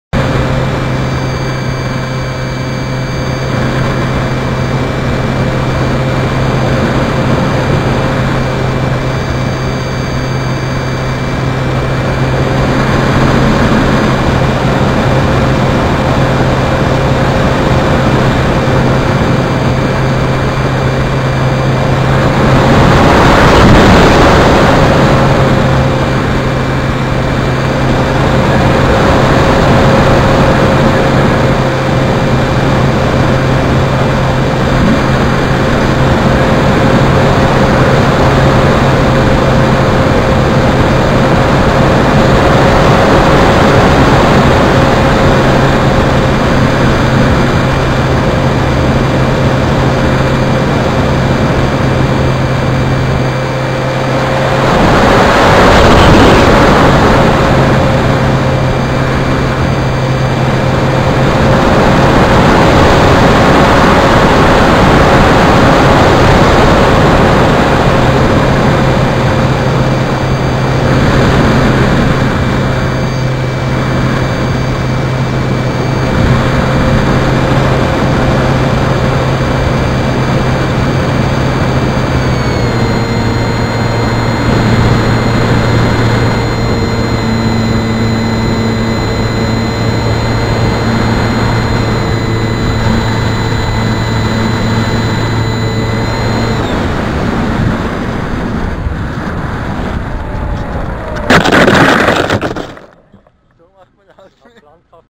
Modelairplane view from Schwändi in Stereoscopic 3D
Recorded with two Mobius action cams model A.